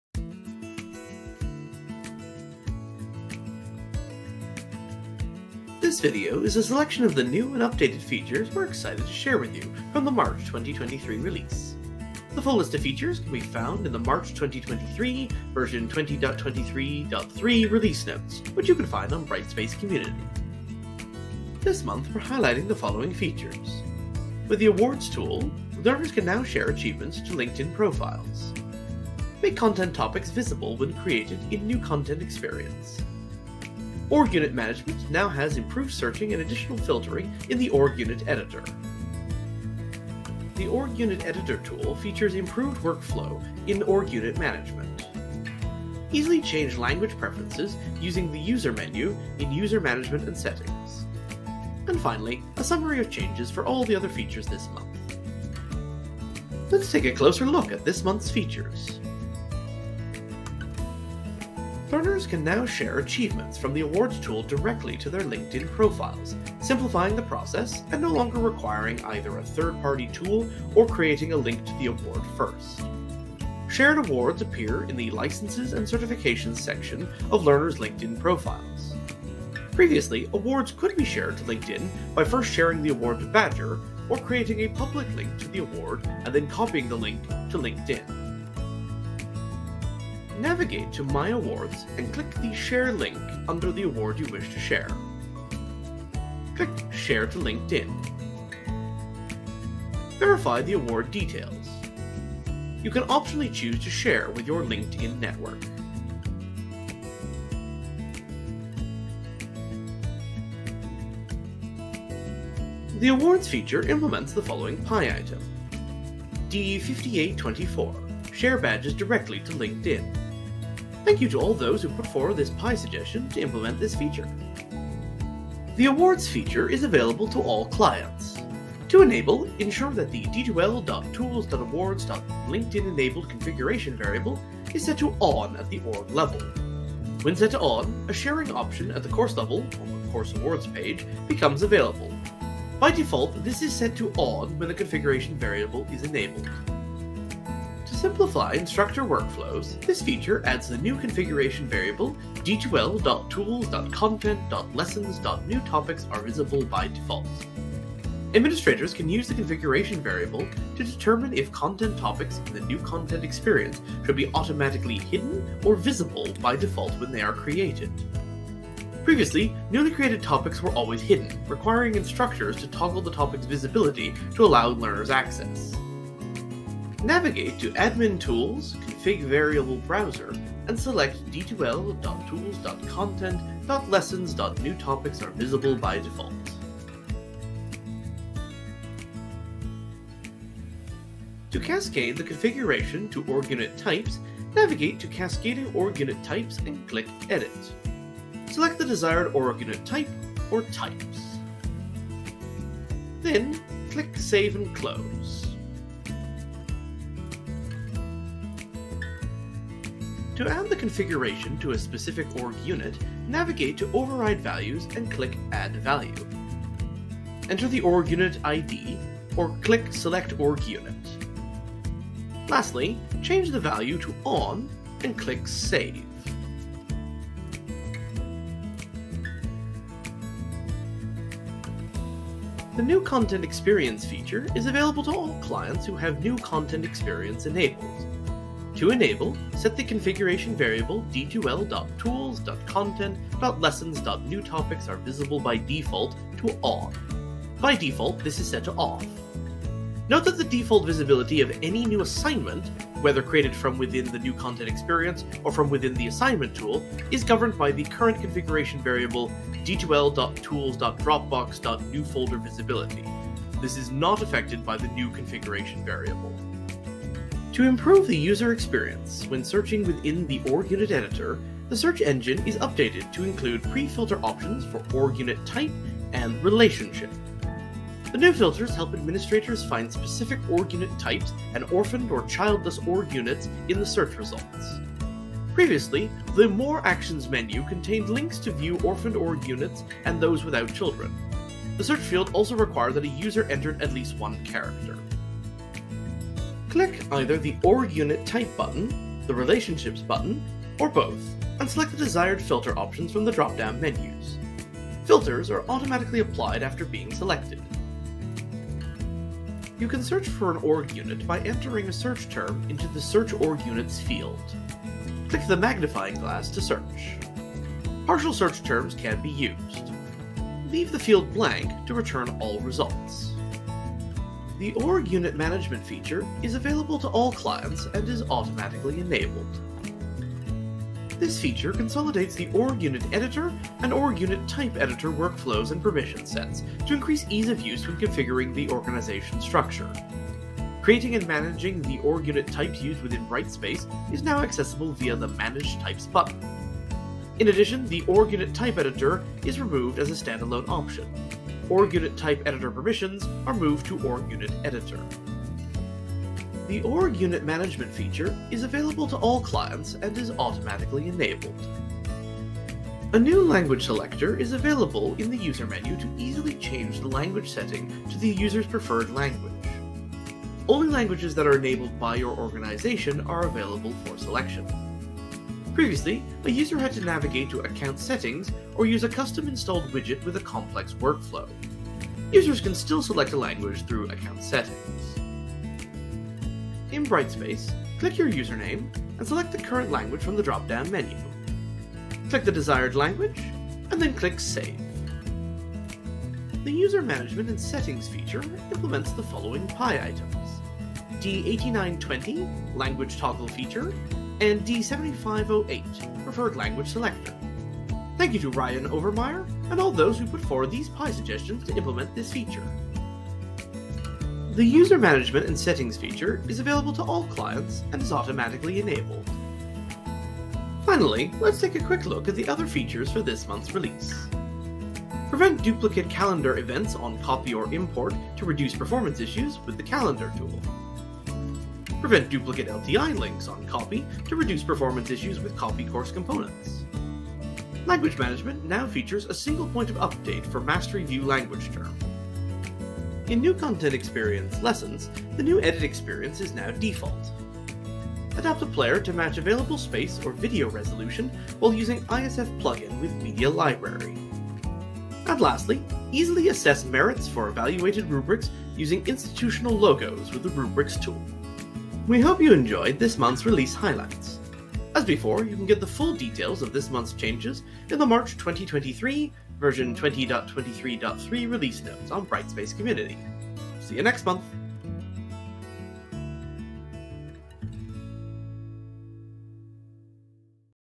March 2023/20.23.3 Release Highlights Video
This video highlights some of the new and updated features of Brightspace 20.23.3. The full list of features can be found in the March 2023 / 20.23.3 Release Notes on Brightspace Community.

-- Release Highlights Contents --
00:00 - Introduction
00:21 - Features List
01:01 - Awards (Share achievements to LinkedIn profiles)
02:27 - New Content Experience (Make content topics visible when created)
04:27 - Org Unit Management (Improved searching and additional filtering in the Org Unit Editor)
05:30 - Org Unit Management (Improved the Org Unit Editor workflow)
06:02 - User Management and Settings (Easily change language preferences using the User Menu)
07:00 - Other Features Summary
07:41 - Outro